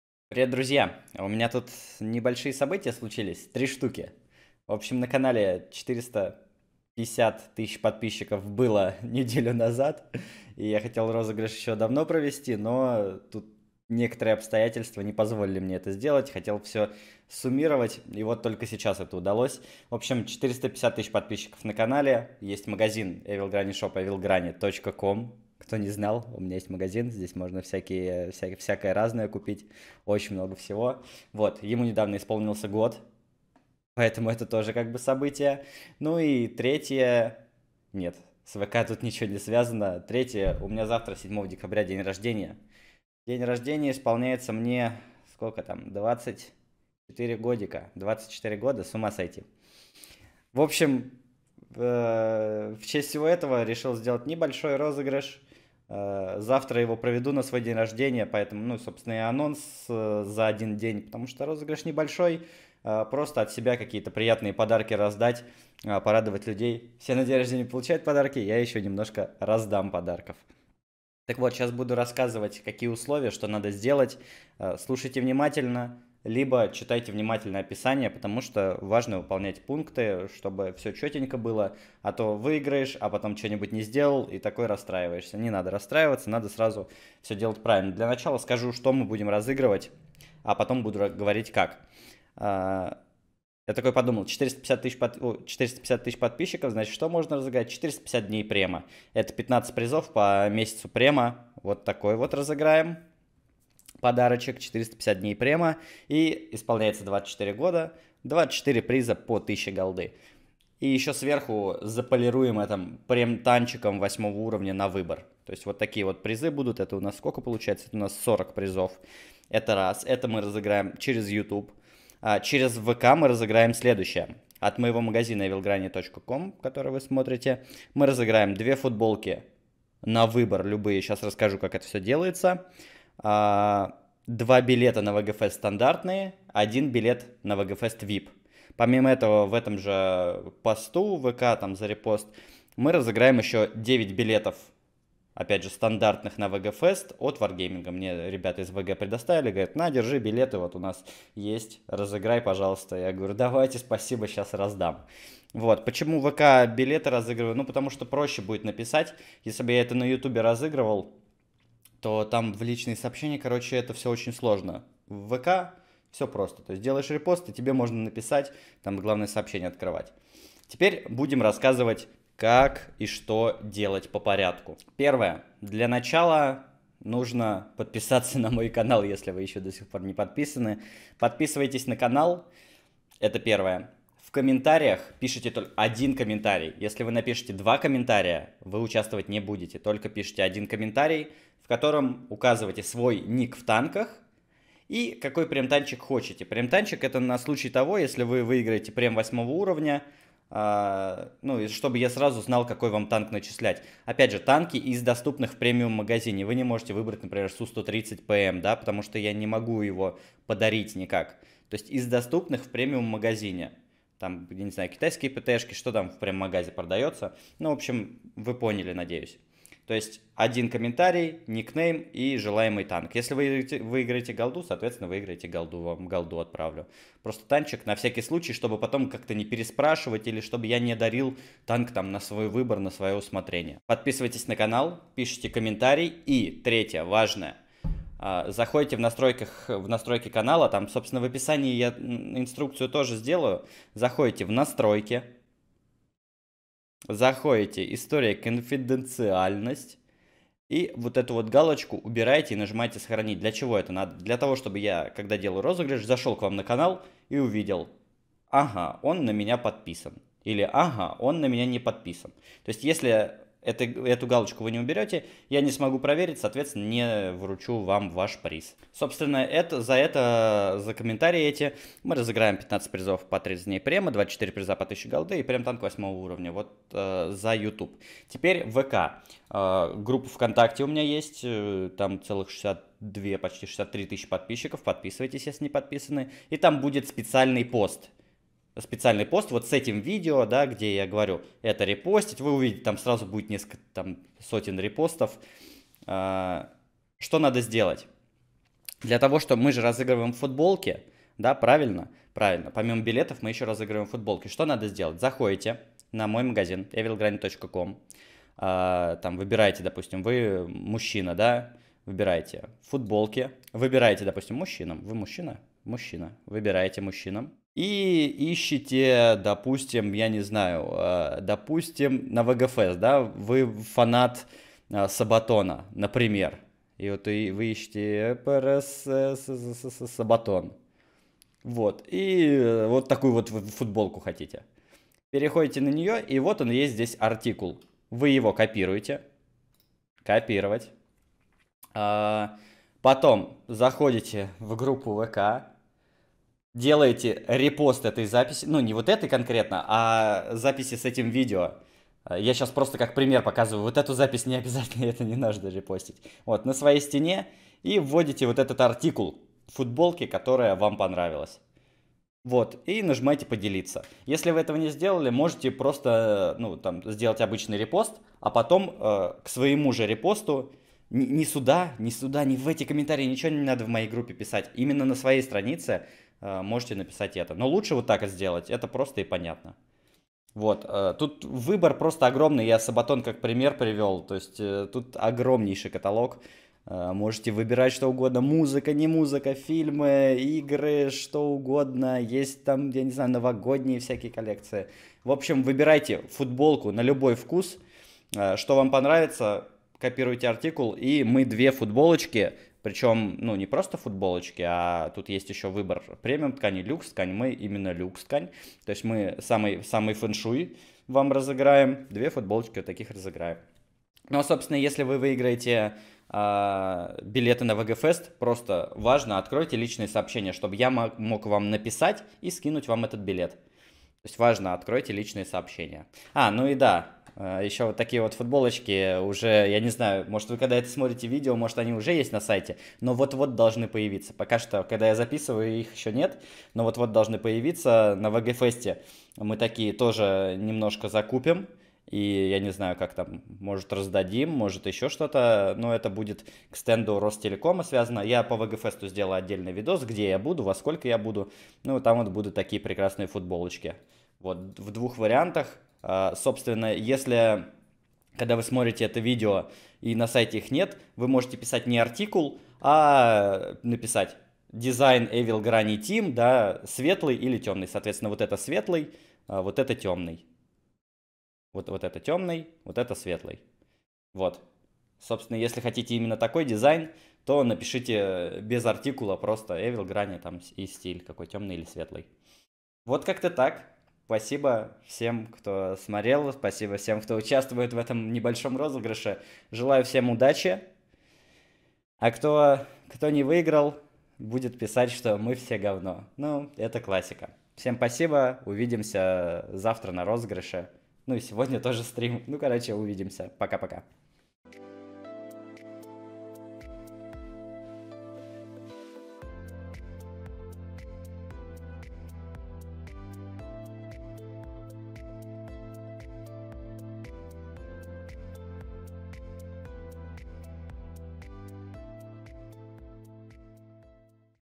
РАЗДАЮ ПОДАРКИ В СВОЙ ДЕНЬ РОЖДЕНИЯ
Правила участия в розыгрыше:
2. Оставь ОДИН комментарий под этим видео в формате: (Игровой ник в танках - Премиум танк 8 уровня из доступных в продаже)
Пример: "EviL_GrannY - ИС-6"

Правила участия в розыгрыше ВК:
1. Зайди на сайт и выбери понравившуюся футболку и скопируй её артикул (например SVP-332738-fut-2)
2. Репостни данную запись:
3. При репосте записи вставь артикул понравившейся футболки.
4. Открой личные сообщения ДЛЯ ВСЕХ, чтобы я смог связаться с тобой и вручить приз.

РЕБЯТА! ОБНОВЛЕНИЕ ИНФЫ ПО РОЗЫГРЫШУ!
Как вы знаете мне предоставил варгейминг 9 билетов на WG Fest. Так вот, среди этих 9 победителей, которые приедут на Фест на стенде Ростелеком разыграют 3 процессора i7 7700K!
Так что розыгрыш теперь ещё жирнее!

Успейте до 20:00 Мск